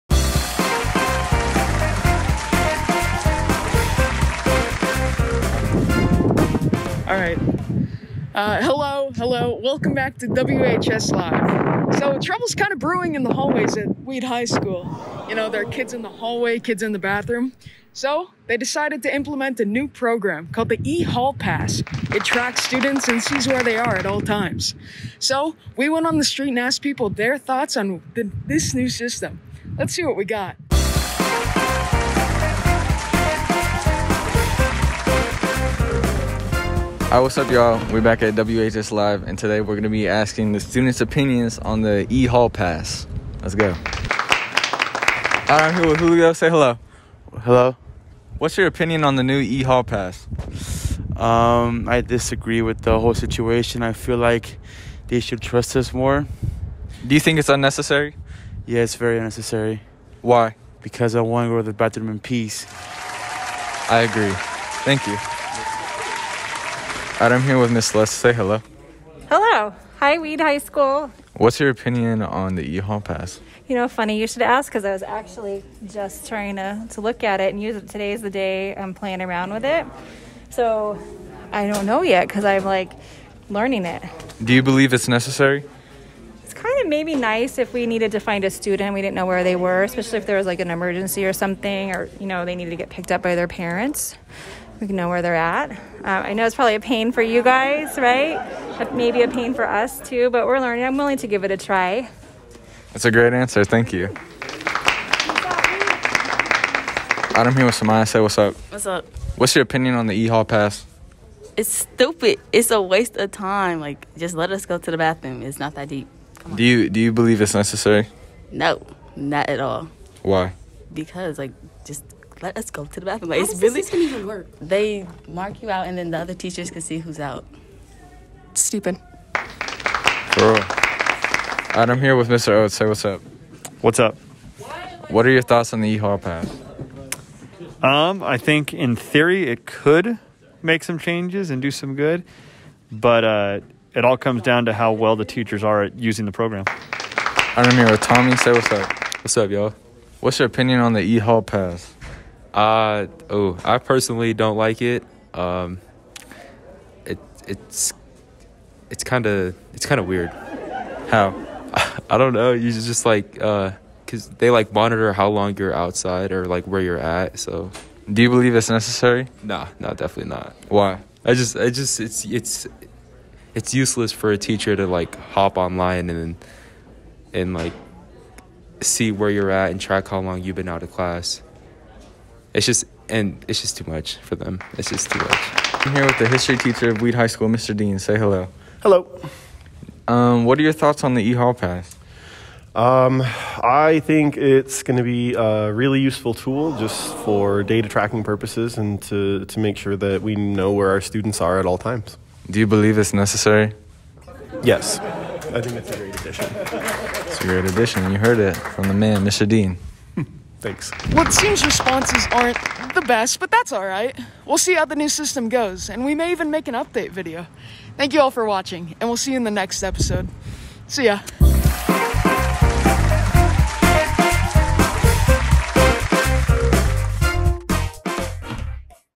WHS Live: The E-Hall Pass
WHS has implemented a new electronic hall pass system called, Ehall Pass. We asked students and teachers to share their thoughts on the new system.